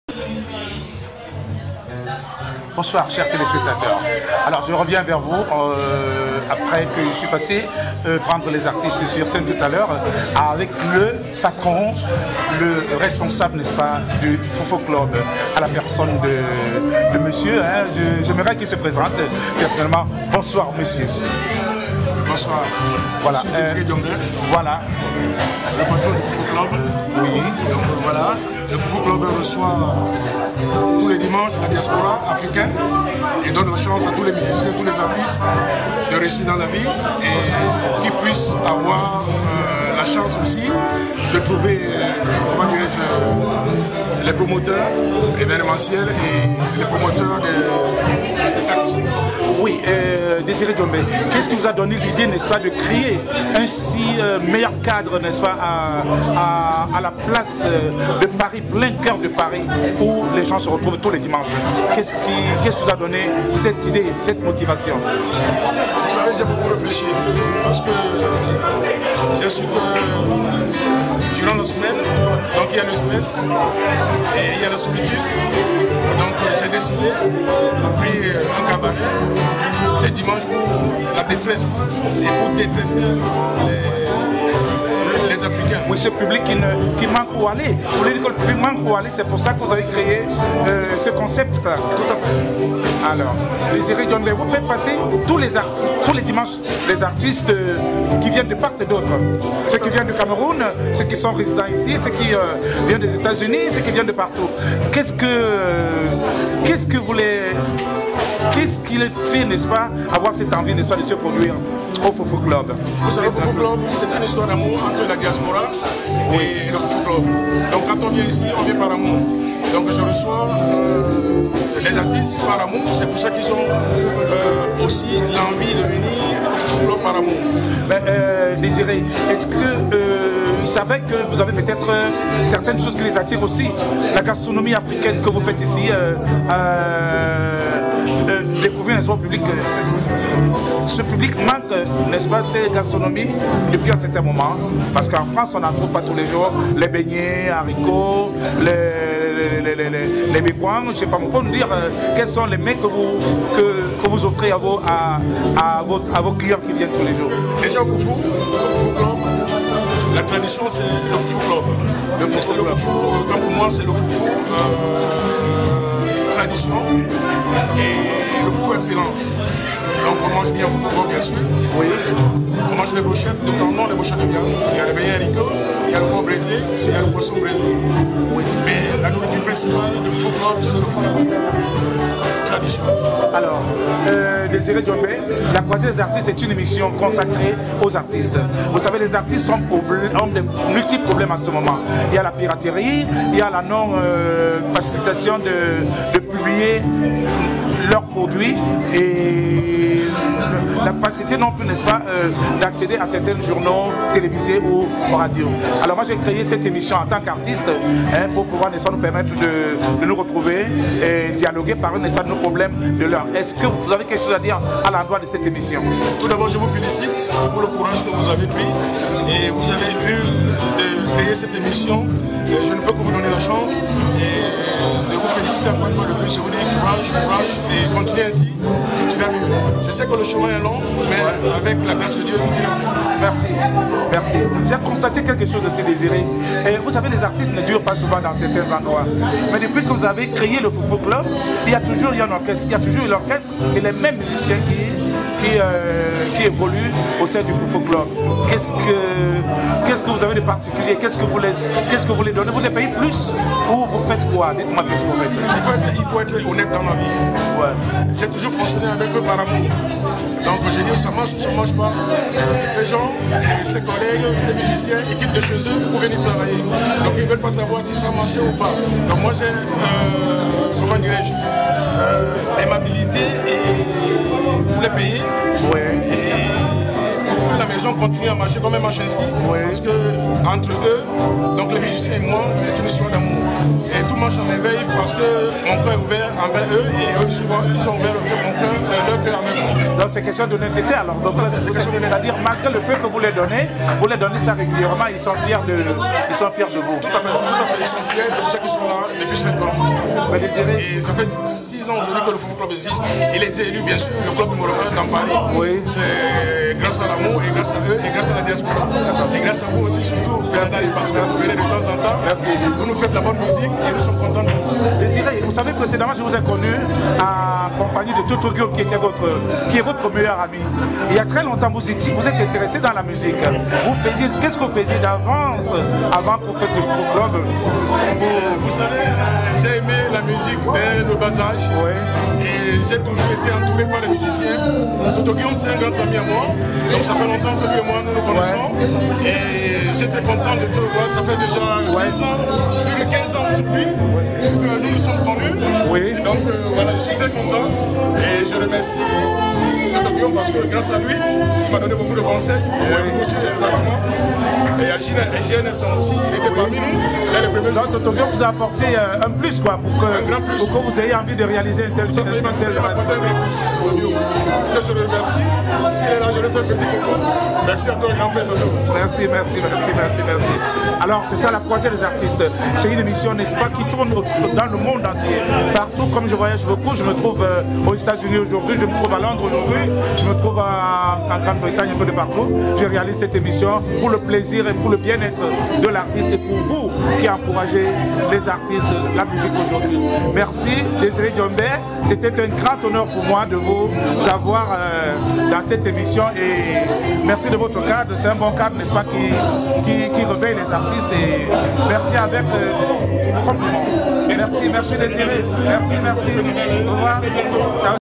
La Croisière Des Artistes Camerounais Présente Mr Désiré Djombe de Foufou Club de Paris
FOUFOU situé au cœur de la ville de Pantin endroit et cadre ou le public de la diaspora passe leurs fin de semaine dans une ambiance de feux à découvrir svp